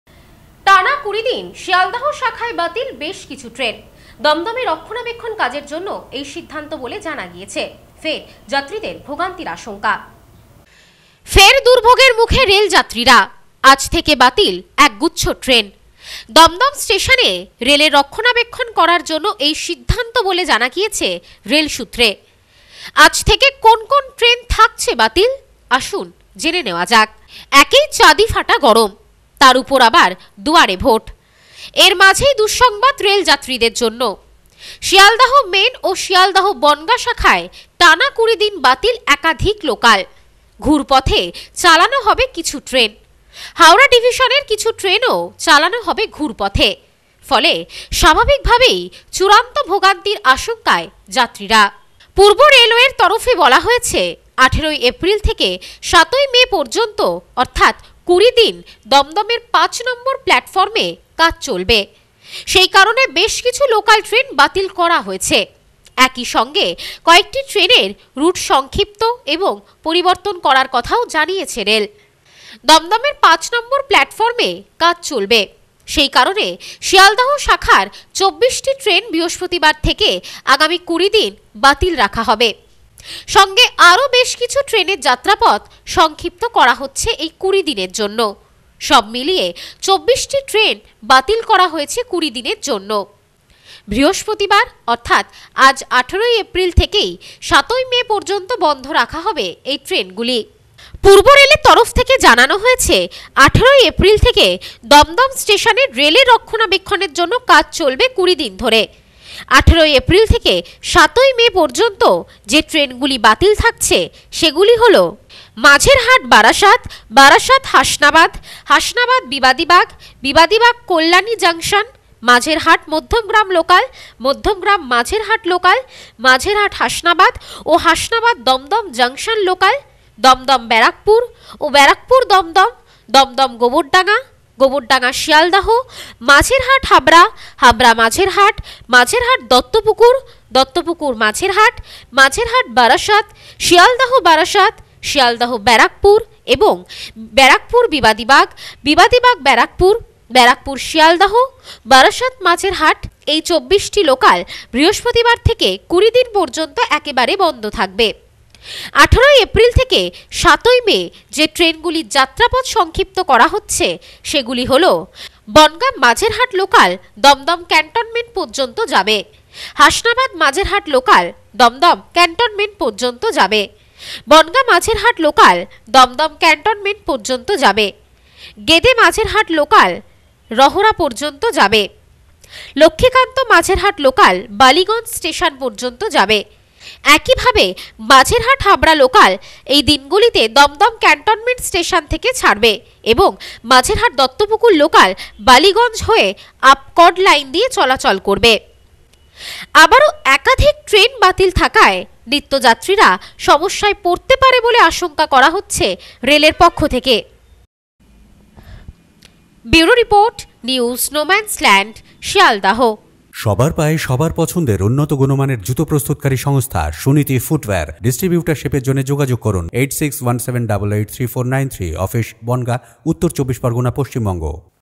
টানা ২০ দিন শিয়ালদহ শাখায় বাতিল বেশ কিছু লোকাল।
টানা ২০ দিন শিয়ালদহ শাখায় বাতিল বেশ কিছু লোকাল।দমদমের রক্ষণাবেক্ষণের কাজ। ফের ভোগান্তির আশঙ্কা যাত্রীদের।